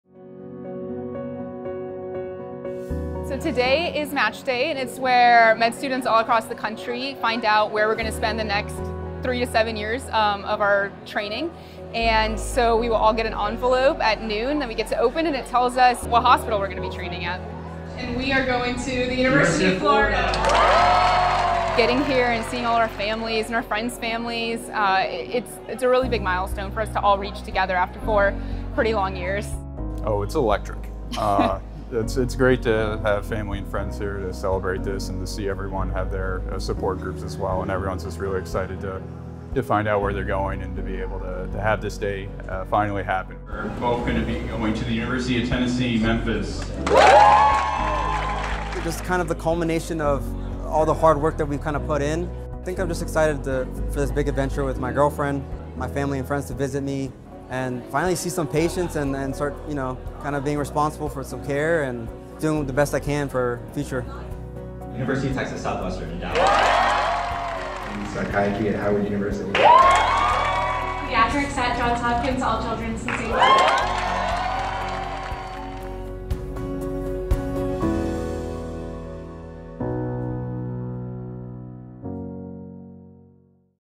UF College of Medicine Match Day 2023
Fourth-year UF medical students learn residency matches during a March 17 ceremony.